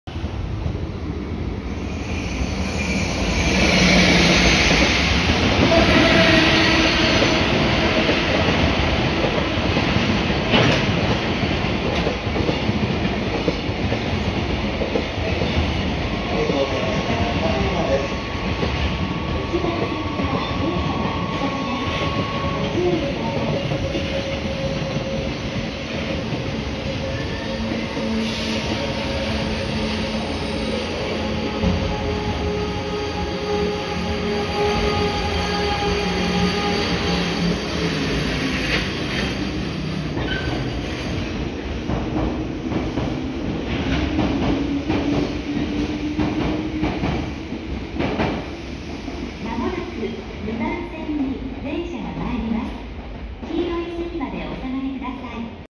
●20200909 EF64形 通過 ＠金山
撮影路線：JR東海 東海道本線
撮影駅：金山
撮影場所：名古屋市熱田区金山町一丁目1-18（名鉄金山駅）
行先：
車両：EF64形
撮影機材：スマホ
MH：ーー
備考欄：